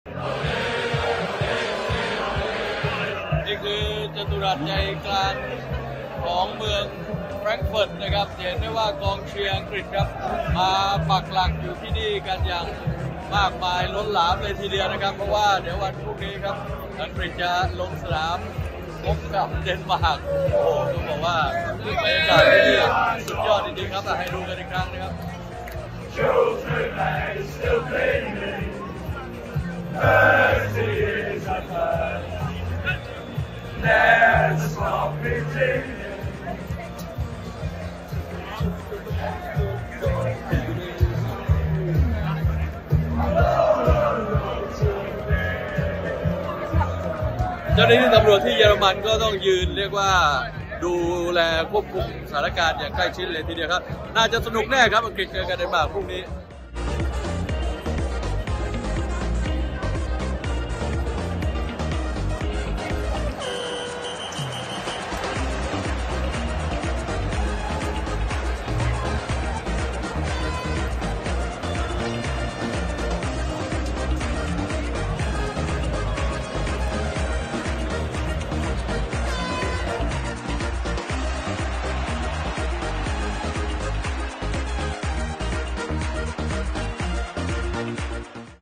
ตำรวจ คุมเข้ม กองเชียร์อังกฤษก่อนเกมปะทะเดนมาร์ก
“บิ๊กจ๊ะ” พาดูบรรยากาศกองเชียร์ทีมชาติอังกฤษ ก่อนเกมที่จะพบกับเดนมาร์ก  ที่กิน ดื่ม ร้องเพลง เต้นอย่างสนุกสนาน ส่วนเจ้าหน้าที่ตำรวจท้องถิ่น ต้องเฝ้าระวังความปลอดภัยเพื่อไม่ให้เกิดเรื่องราววุ่นวาย